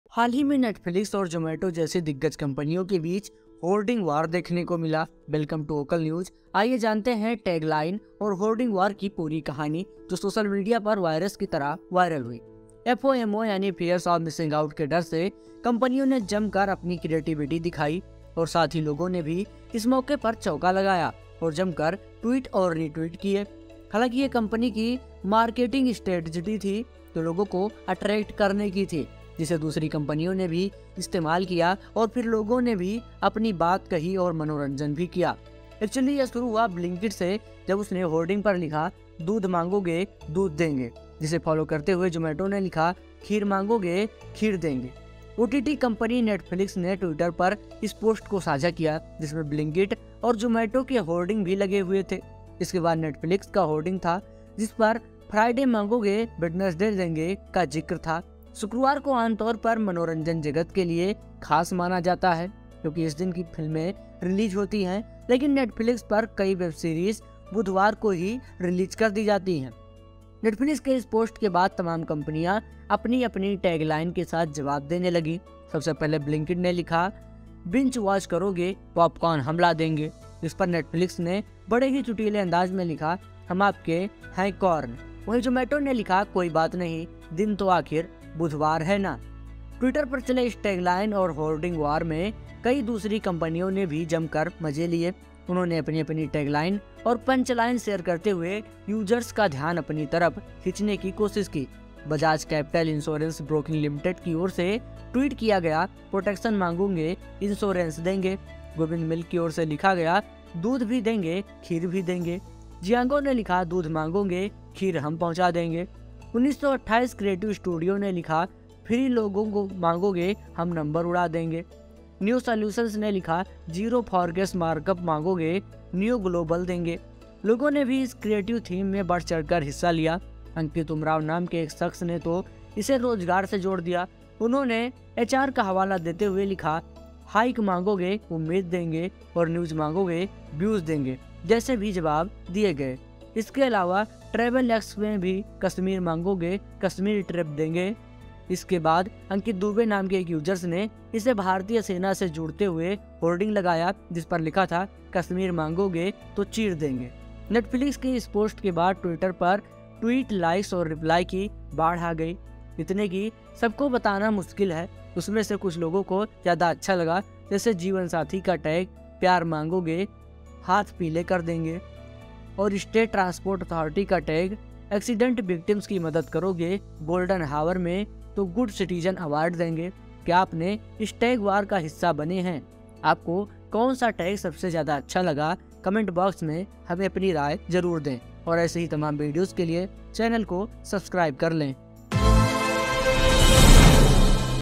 जाने क्या है Billboard War? जिसके लिए नेटफिलिक्स, जोमैटो जैसी बड़ी कंपनियाँ लड़ रही थीं
» हाल ही में ऐसा कुछ बड़ी-बड़ी कंपनी netflix और जोमैटो जैसे दिग्गज कंपनियों के बीच होल्डिंग वार देखने को मिला।
आइए जानते हैं टैगलाइन और होर्डिंग वार को पूरी कहानी जो सोशल मीडिया पर virus की तरह viral हुआ। «

» वोकल न्यूज़ से आप इन सोशल मीडिया चैनल्स पर भी जुड़ें-